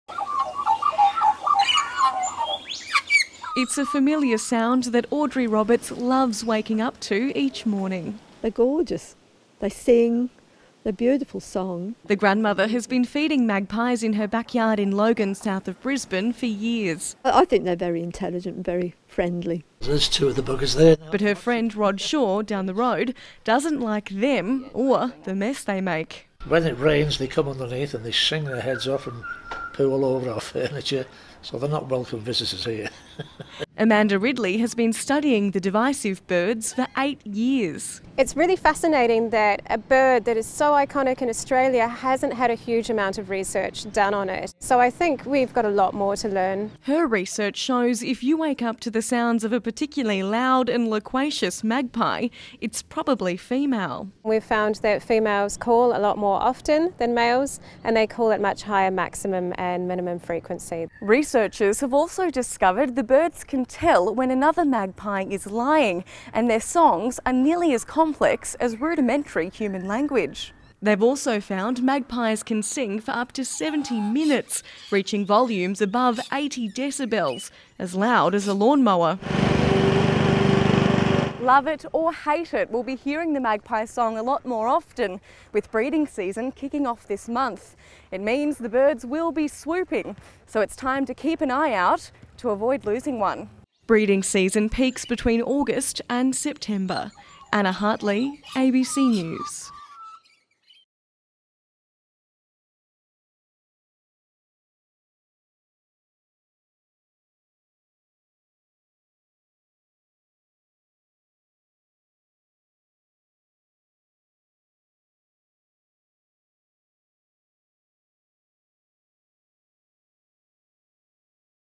Female magpies are the loudest and chattiest of the species, research shows | ABC News
Australia would be a very different place without the distinctive dawn chorus of the magpie, and now research shows females are more chatty than males.

Anna Hartley reports.